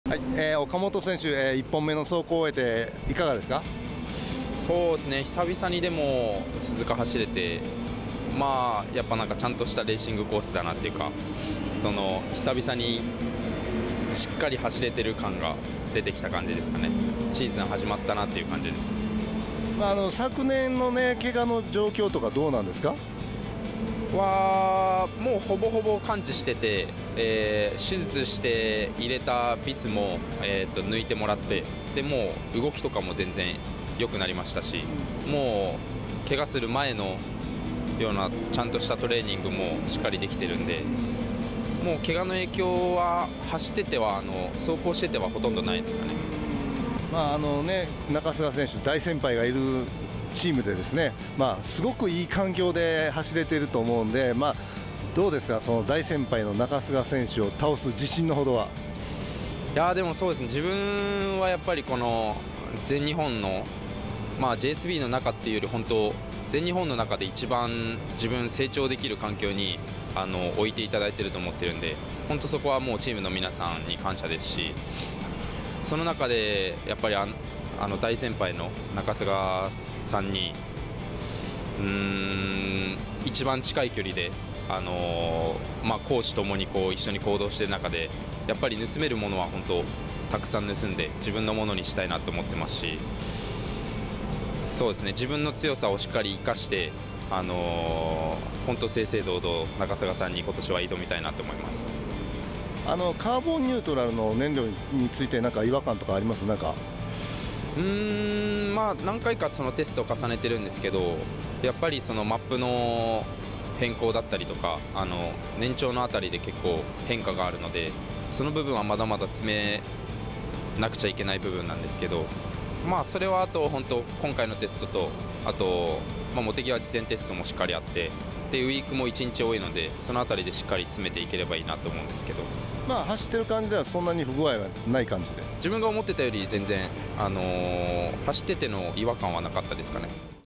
YAMAHA FACTORY RACING TEAM/岡本 裕生 選手
YAMAHA FACTORY RACING TEAM/岡本 裕生 選手インタビュー
岡本 裕生 選手（YAMAHA FACTORY RACING TEAM）の開幕前インタビューです。